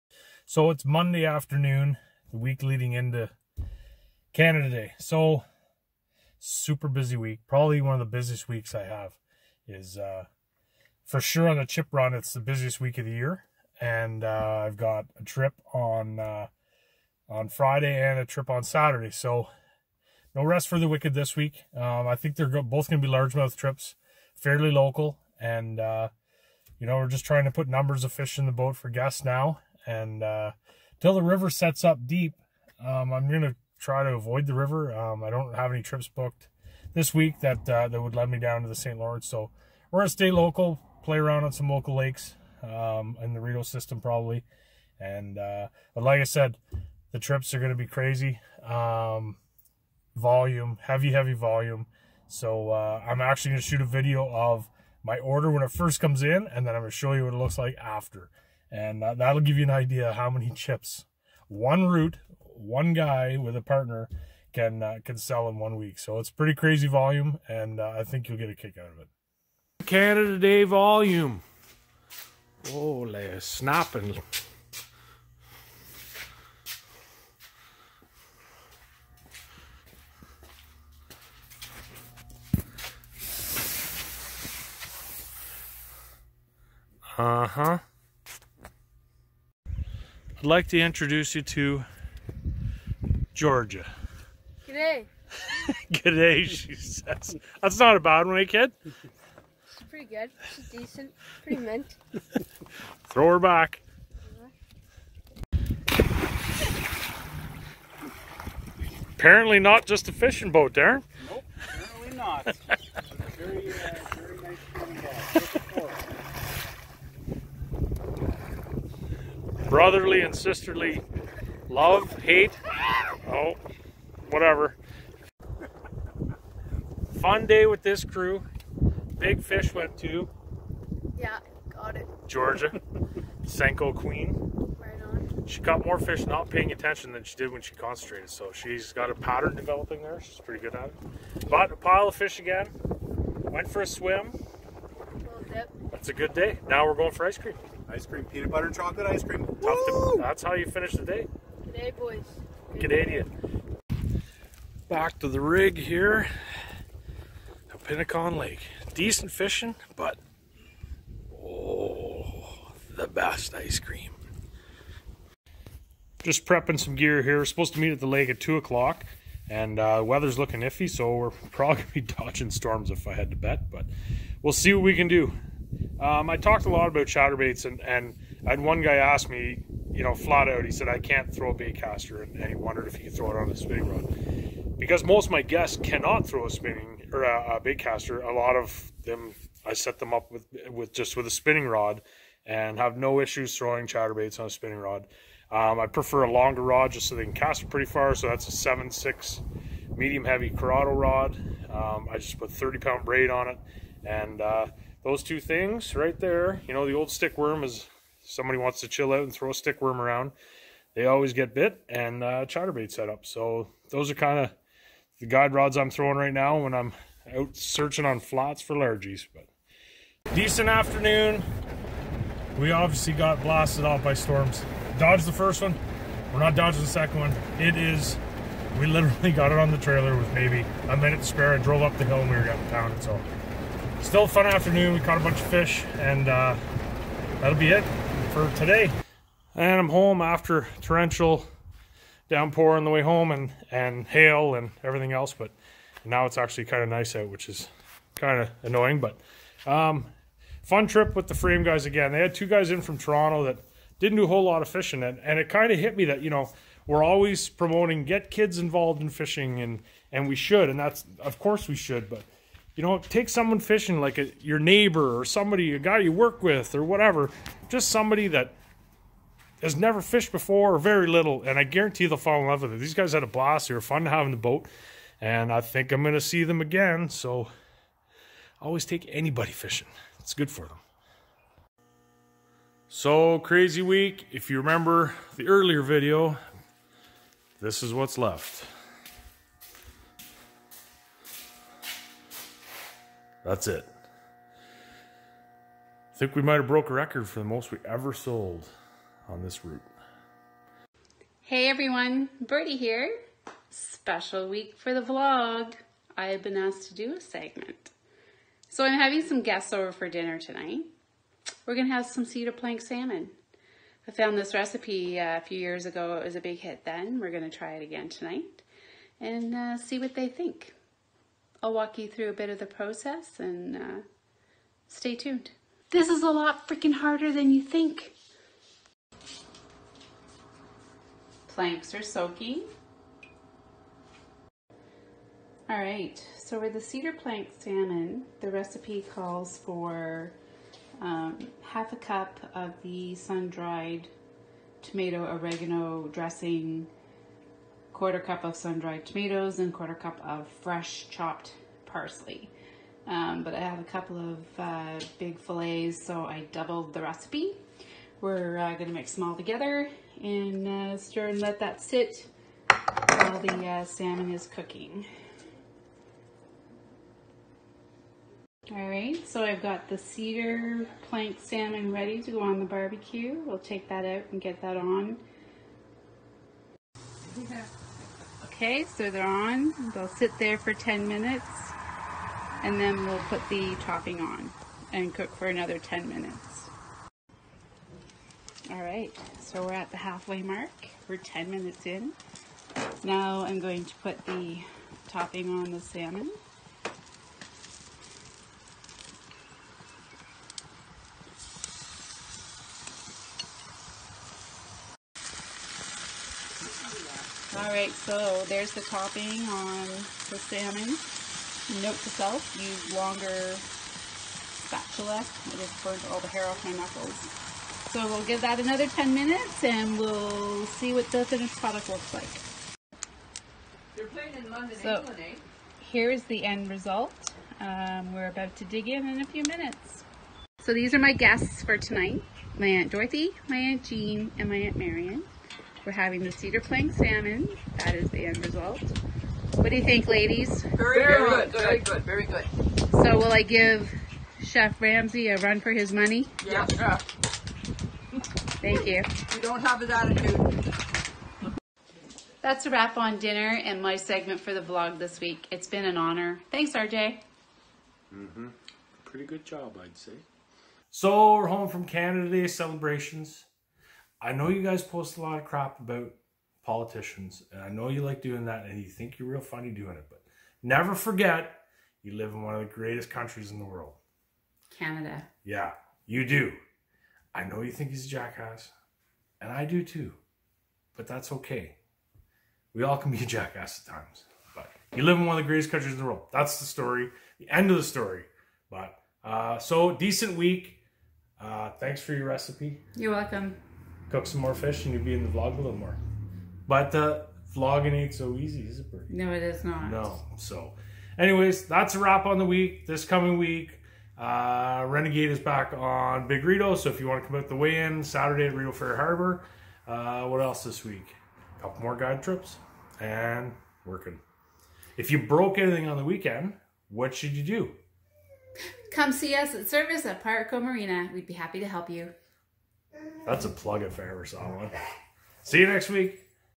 VLOG 20 - Largies, Cedar planked Salmon and lots of Chips!
More Largies, a step by step cedar planked Coho salmon recipe by Birdee, a crazy busy week on the Chippies and celebrating our fine country!